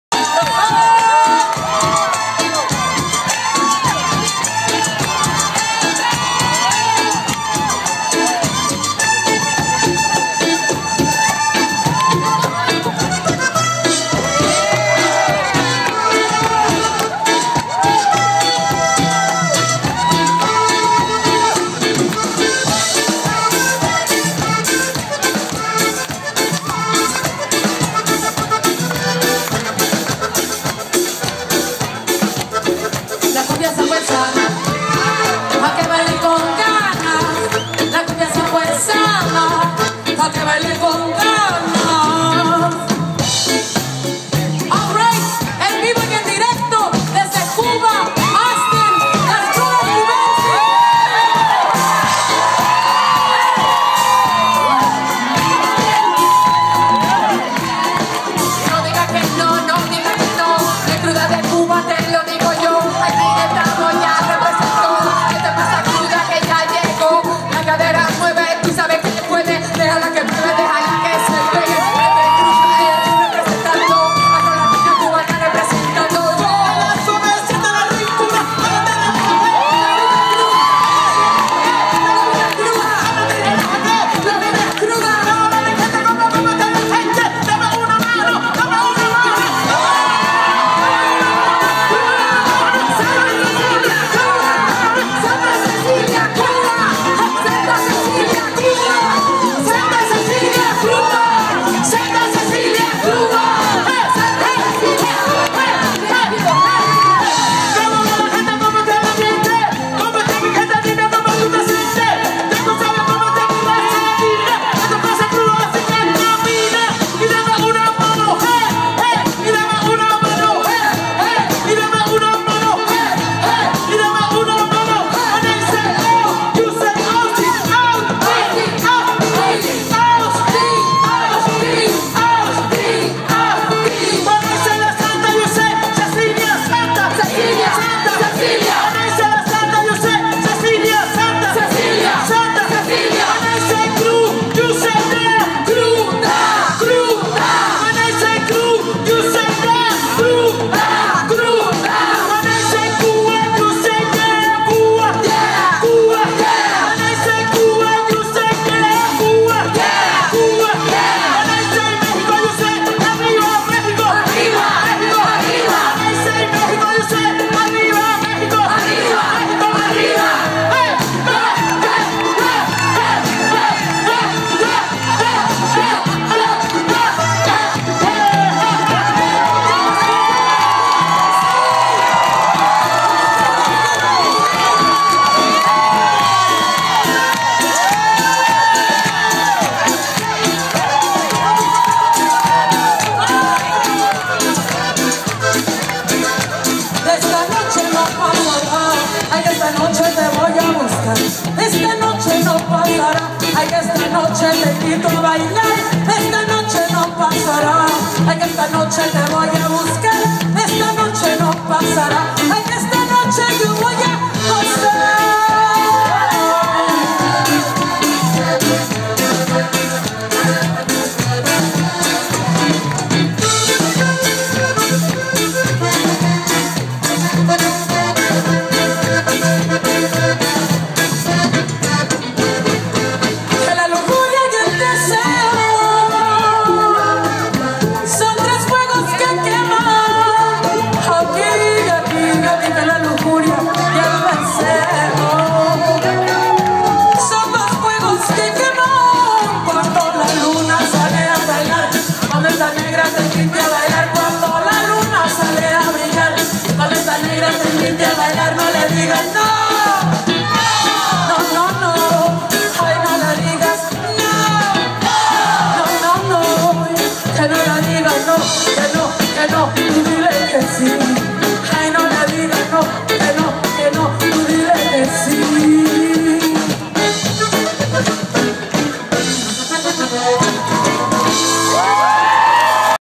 La Santa Cecilia & Las Krudas - Pachanga Fest - Austin Texas
La Santa Cecilia consists of guitarist Gloria Estrada, accordionist and requintero Jose Carlos, bassist Alex Bendana, percussionist Miguel Ramirez, drummer Hugo Vargas